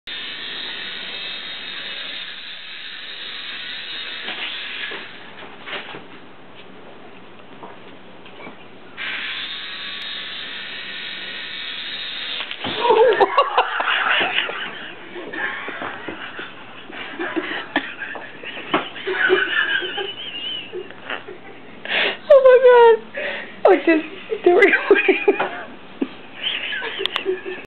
Toy Helicopter vs German Shorthair pup
Our puppy Cheyenne watching a toy helicopter. When the attack occured